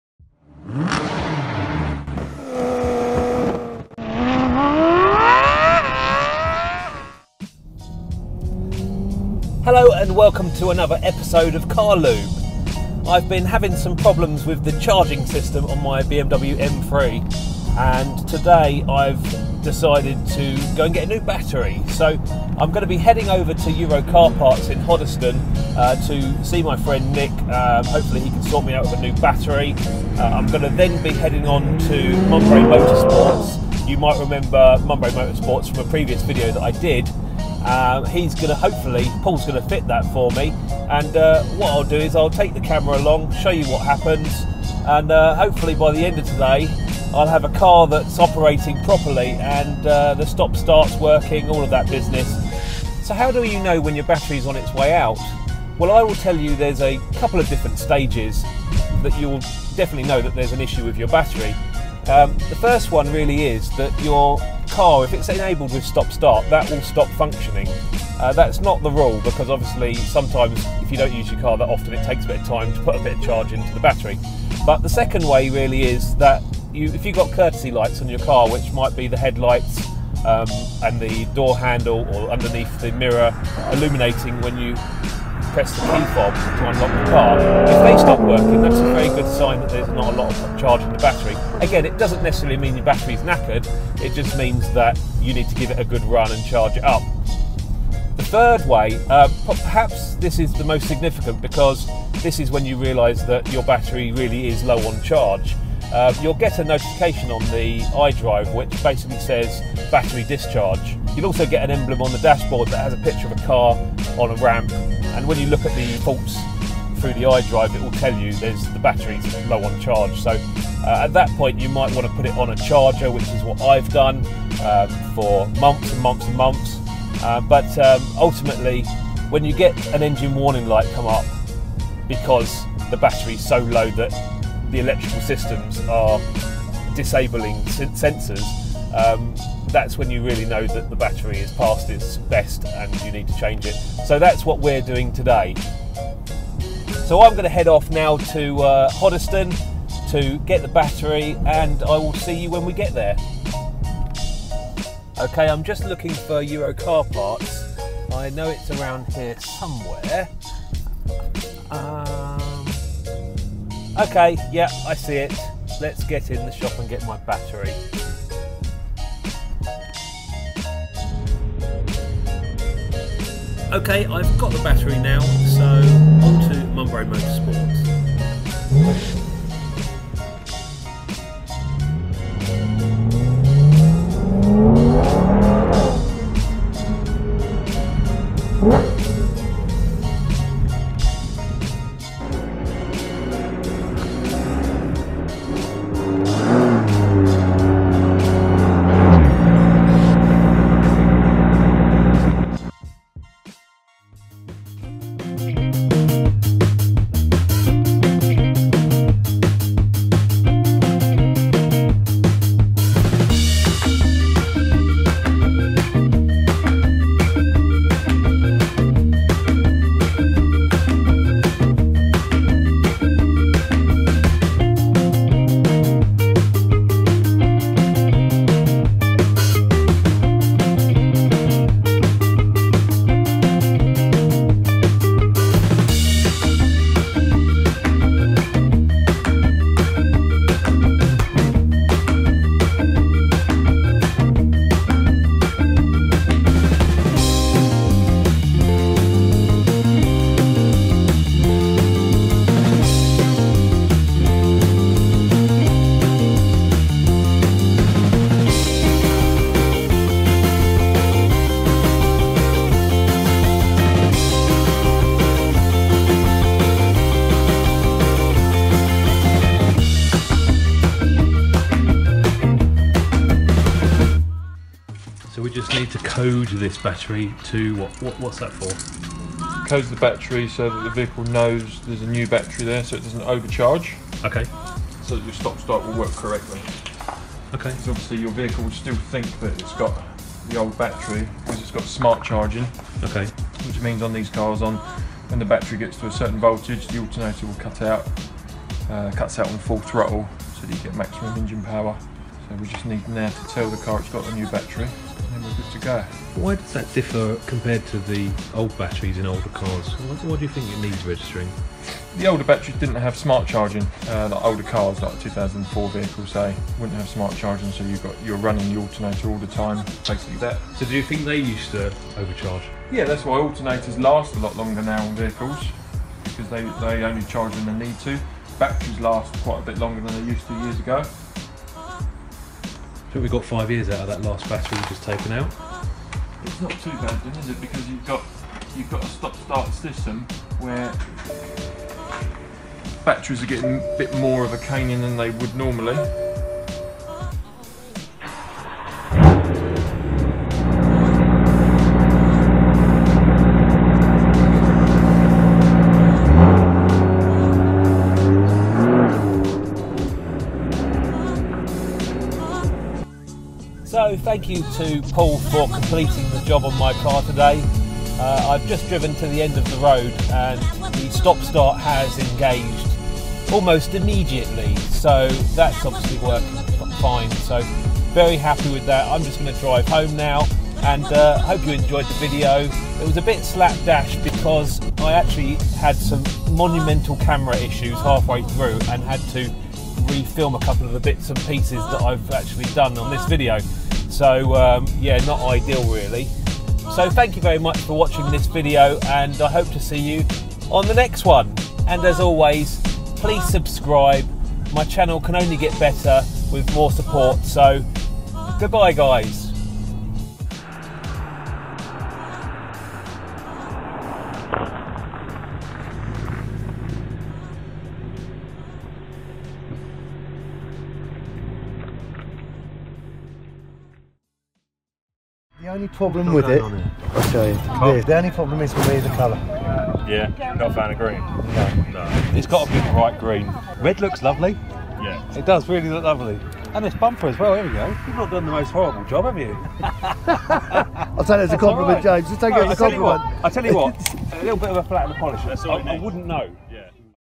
What's wrong now? BMW E93 M3 Battery install Vlog
More maintenance on My BMW M3 today, follow the Vlog

Please feel free to follow me on other social media networks.